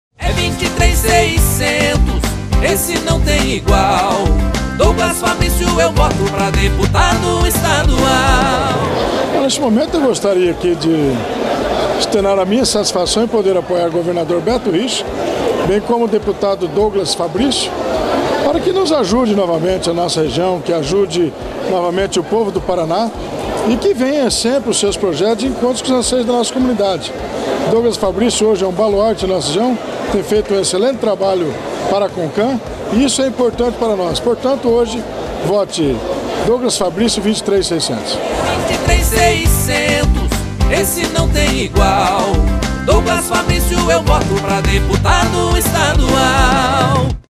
Prefeito Baco também apoia Douglas Fabrício 23600 Deputado Estadual
Prefeito de Ubiratã, Haroldo "Baco" Duarte também apoia Douglas Fabrício 23600 Deputado Estadual. Confira!